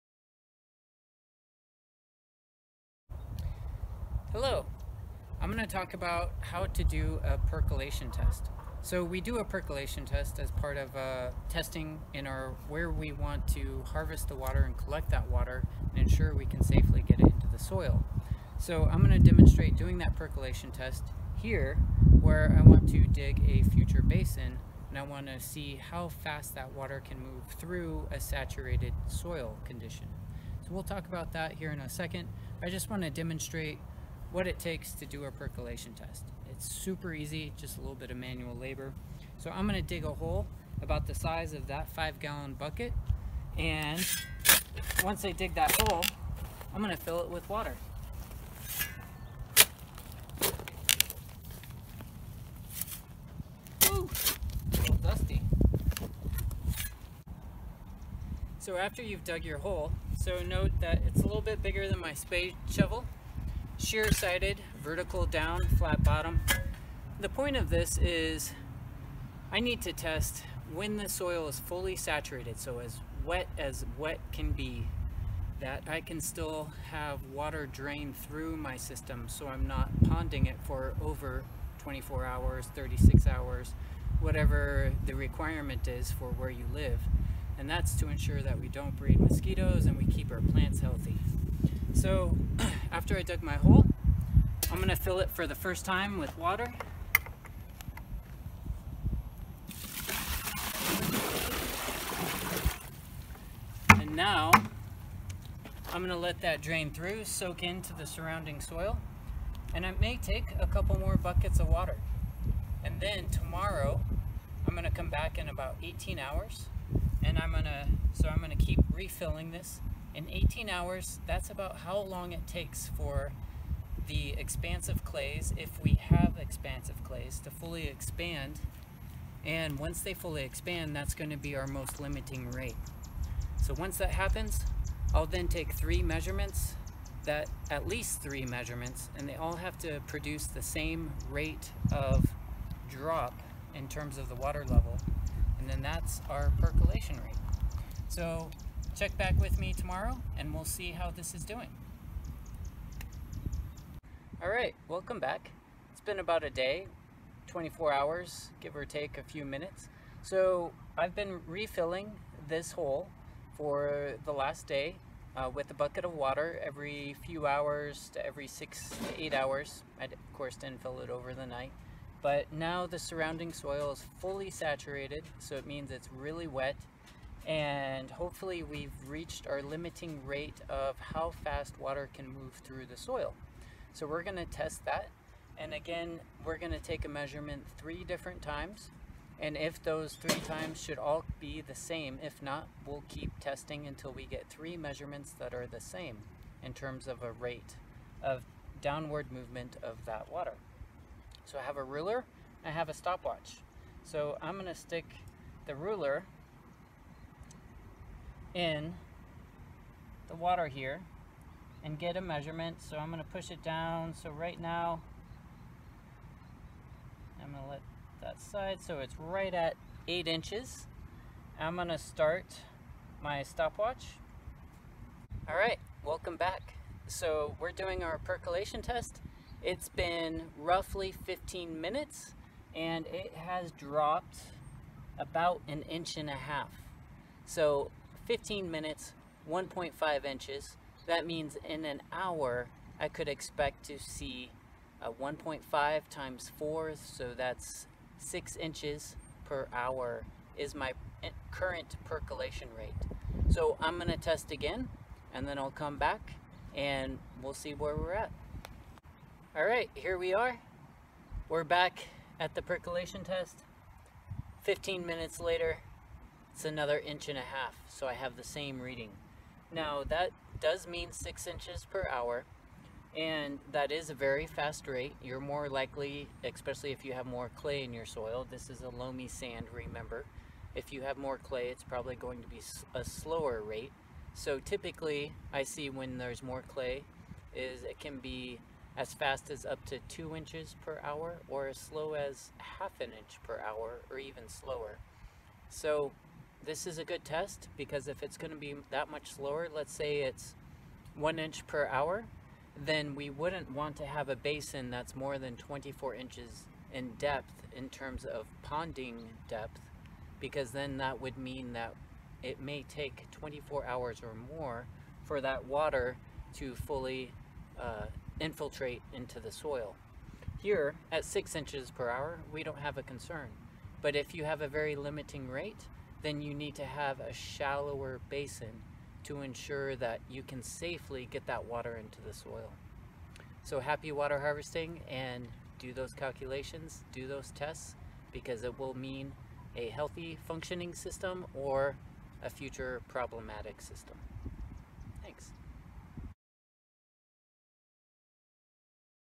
How to conduct a soil percolation test
Learn how to do a simple soil percolation test to help you design your water harvesting basin (rain garden) or greywater system.

Visit Watershedmg.org/FieldStudies to learn more about sustainable desert living, including on building healthy soils!